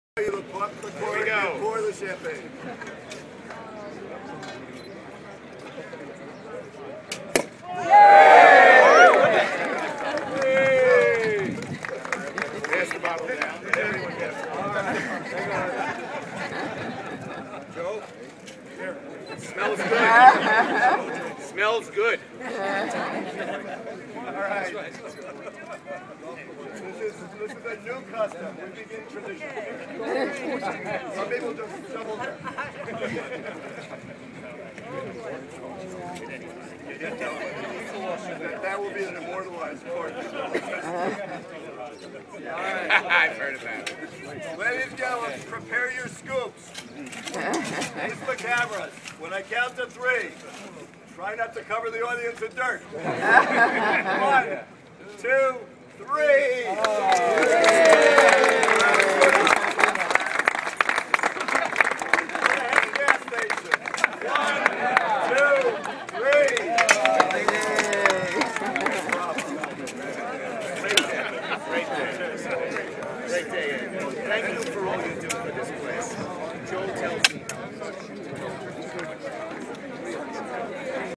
Groundbreaking of College Town at the University of Rochetser
The official groundbreaking for the College Town project at the University of Rochester took place on Thursday, May 2, 2013.